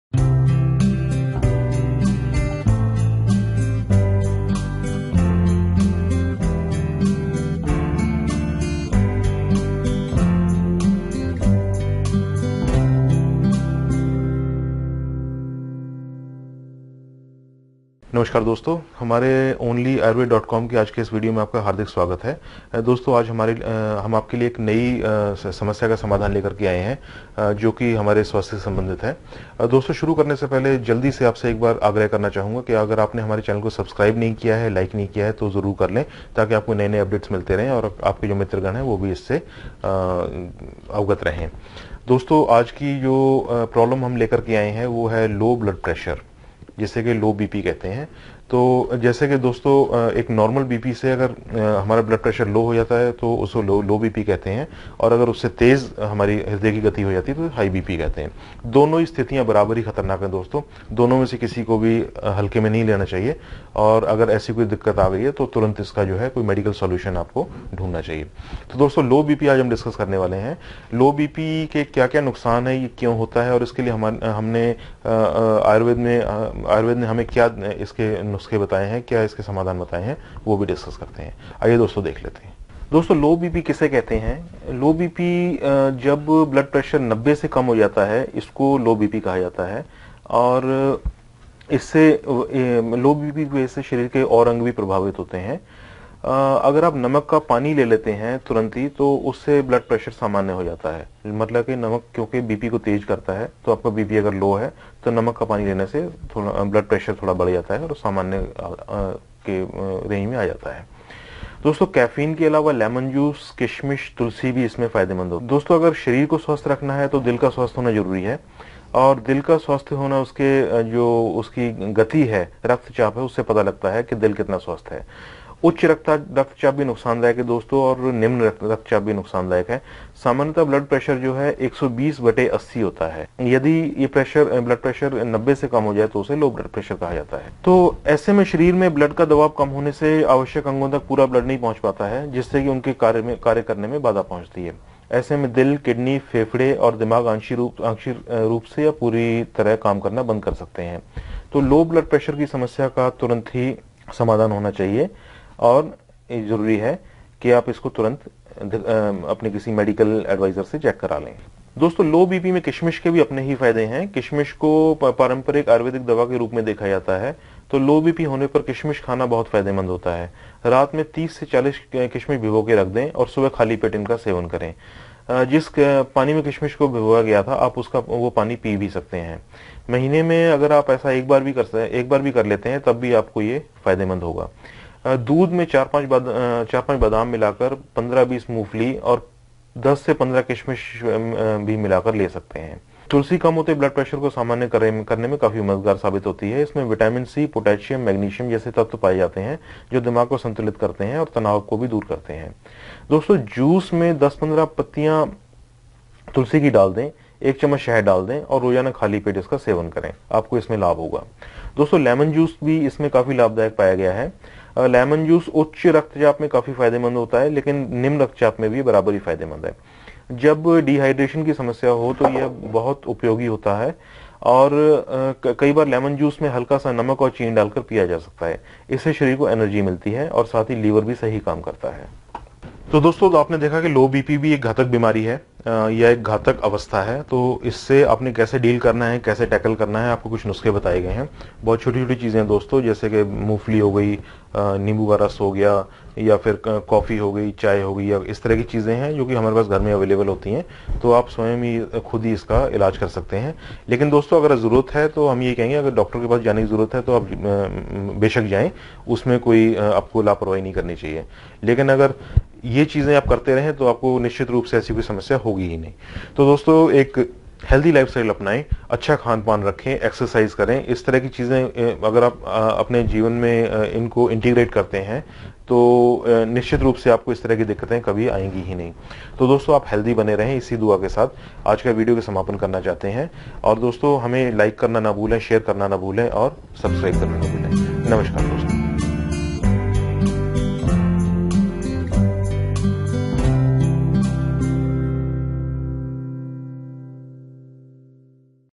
निम्न रक्तचाप के लिए बेहद असरदार है ये घरेलु उपाय
निम्न रक्तचाप के लिए बेहद असरदार है ये घरेलु उपाय / Easy and effective home remedies for low BP